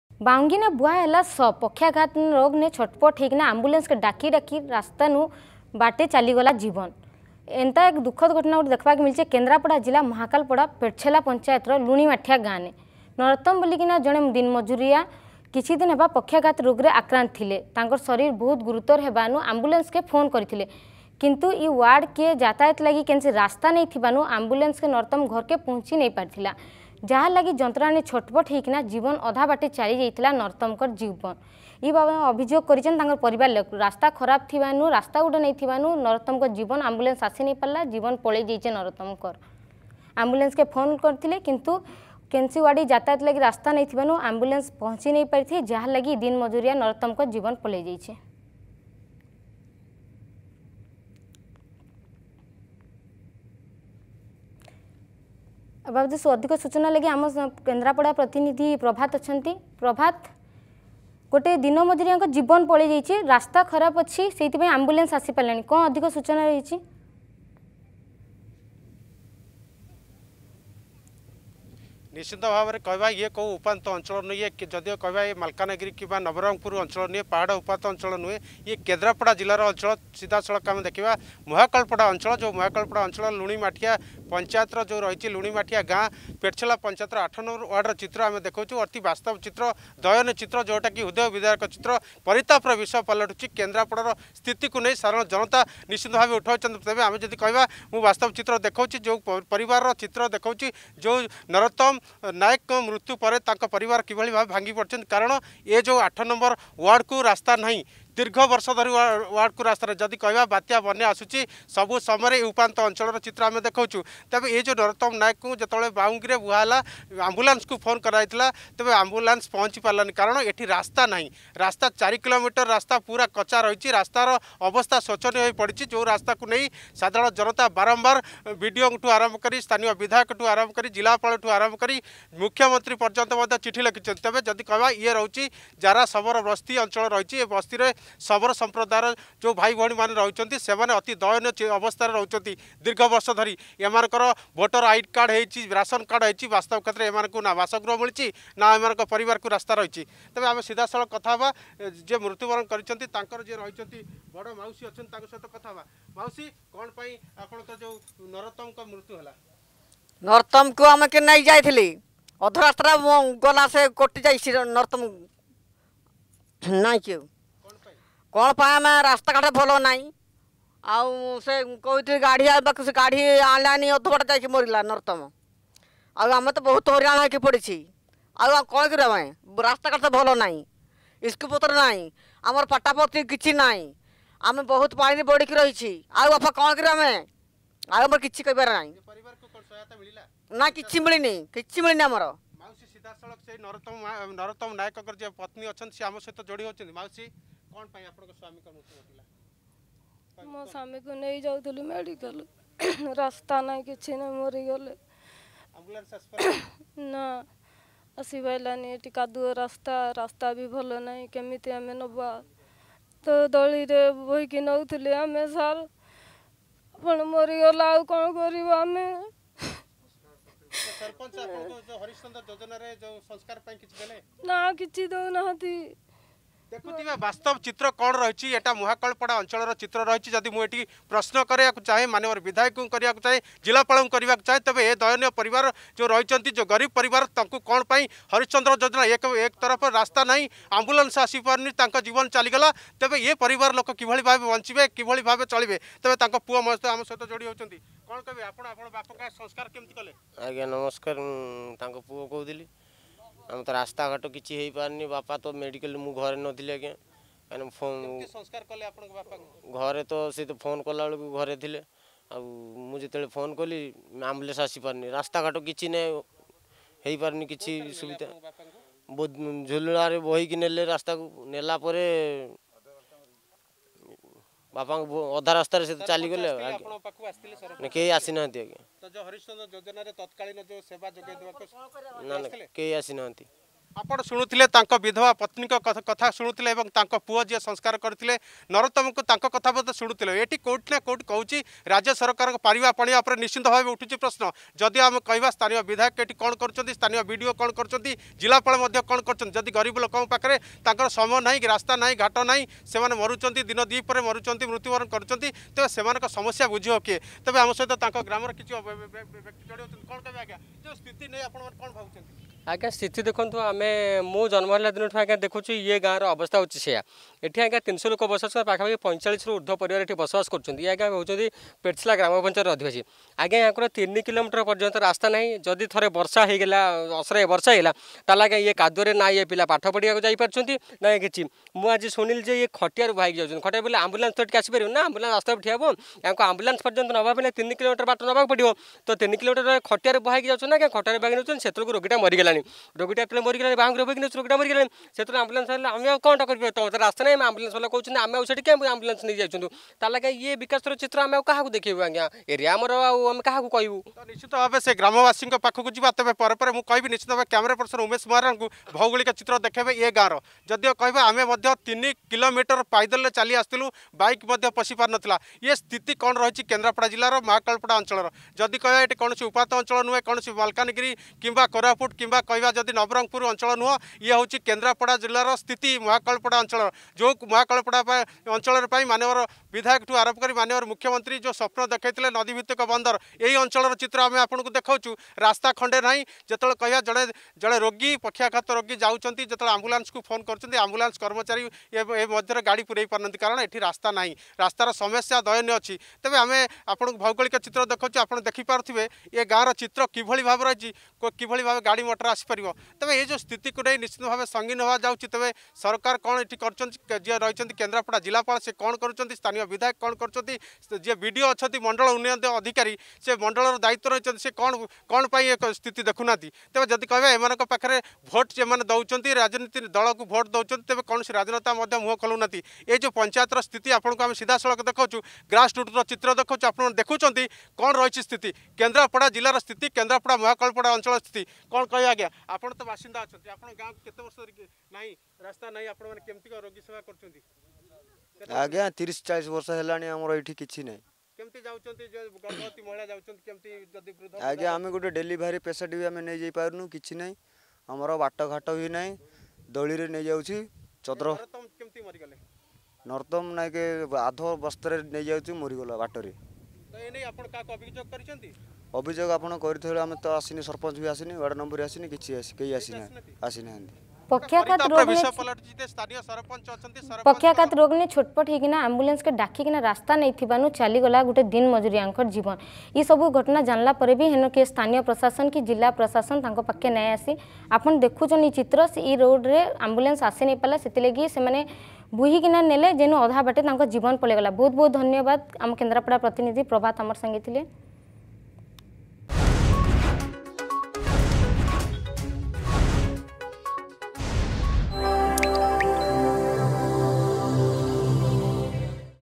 ବାଉଙ୍ଗିରେ ବୁହା ହେଲା ଶବ
ବାଉଙ୍ଗିରେ ବୁହା ହେଲା ଶବ ।  ପକ୍ଷାଘାତରୋଗରେ ଛଟ ପଟ ହୋଲ ଆମ୍ବୁଲାନ୍ସ କୁ ଡାକି ରାସ୍ତା ନଥିବାରୁ ବାଟରେ ଚାଲିଗଲା ଜଣେ ଦିନ ମଜୁରିଆଙ୍କ ଜୀବନ ।